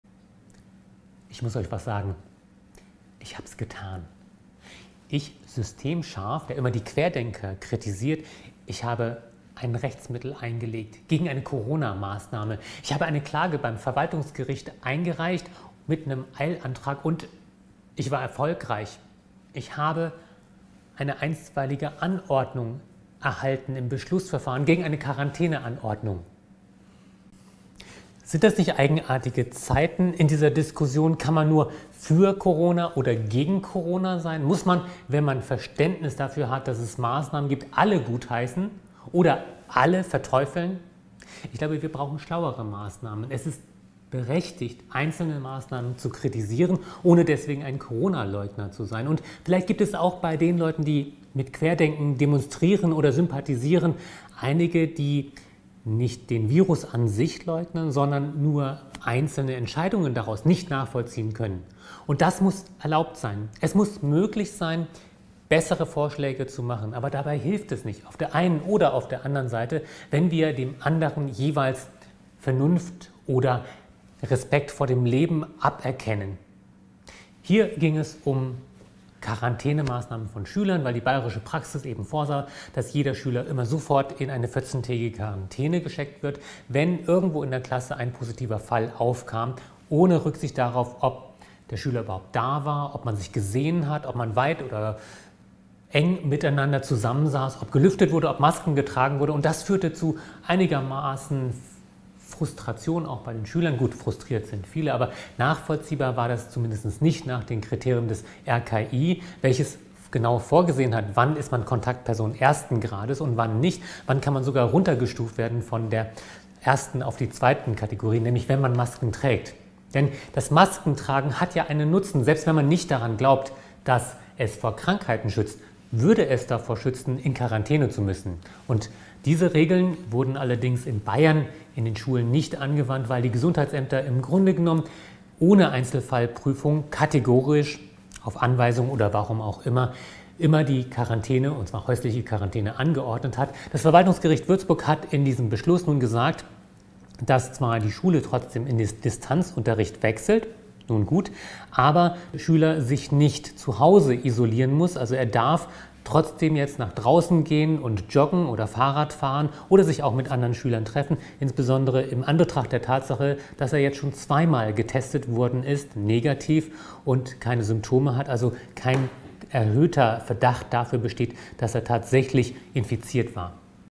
Darf man gleichzeitig Querdenker und Gesundheitsämter kritisieren? Gericht hebt Quarantäne auf.
Darf man staatliche Corona-Maßnahmen in Frage stellen und gegen eine Anordnung des Gesundheitsamtes vorgehen, wenn man die Pandemie ernst nimmt? Ich habe es getan, weil wir nicht einfach nur mehr Maßnahmen brauchen, sondern schlauere und gezieltere Maßnahmen.

Warum muss man eigentlich immer für alle oder gegen alle Maßnahmen sein? Gibt es denn gar keinen Zwischenweg? Ich würde gerne Unsinn auf beiden Seiten als solchen benennen.

Das Verwaltungsgericht Würzburg hob auf unseren Antrag eine Schüler-Quarantäneanordnung auf und stellt sich damit gegen die Bayerische Praxis der unbedingten Schulklassen-Quarantäne bei einem positiven Testfall ohne jedoch den Distanz-Unterricht aufzuheben - ein Kompromiss, mit dem man leben kann. Kurz zuvor hatte schon der Bayerische Verwaltungsgerichthof entschieden, dass das Gesundheitsamt im Einzelfall ein Ermessen ausüben muss und nicht jeden Fall gleichbehandeln kann.

Das heißt nicht - wie es die Anwälte für Aufklärung behaupten - dass die PCR-Tests falsch sind, es heißt nicht, dass jede Quarantäne verfassungswidrig ist, sondern es bedeutet, dass es einen Unterschied macht, ob die Schulen Hygienepläne durchsetzen, Schüler Masken tragen, Lüften und wo sie sitzen, aber auch, ob sich ein Schüler testen lässt.